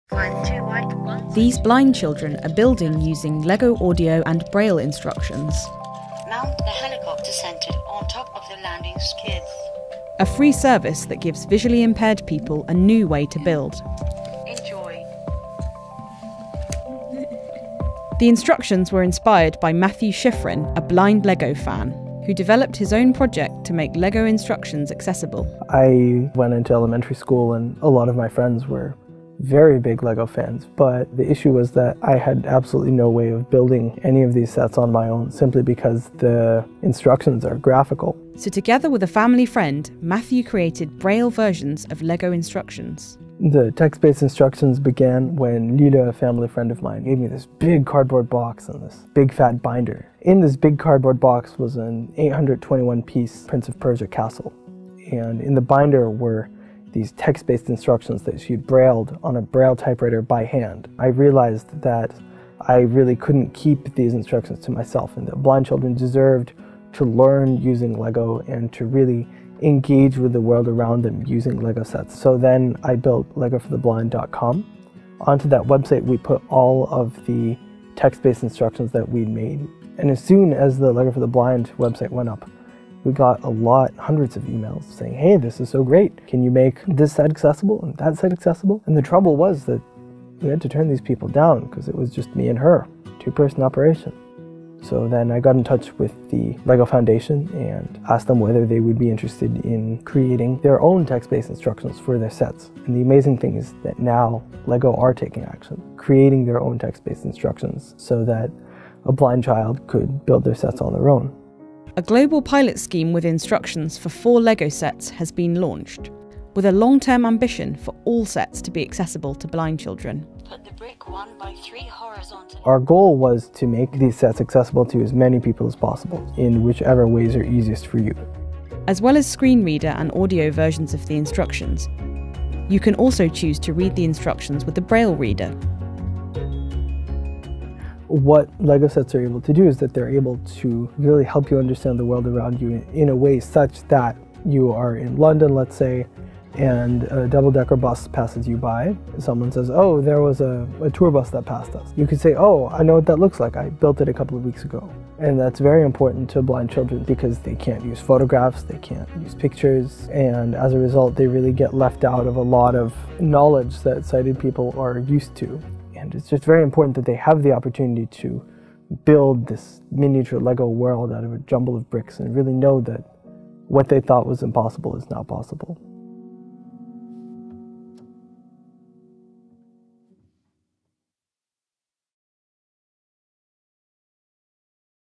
LEGO Audio & Braille Building Instructions test phase launches
While those who can see might take LEGO instructions for granted, those with a visual impairment have not had much choice in how they build LEGO sets – generally, they have needed helped. Now, inspired by blind entrepreneur Matthew Shifrin, the LEGO Group is trialling new LEGO Audio & Braille Building Instructions.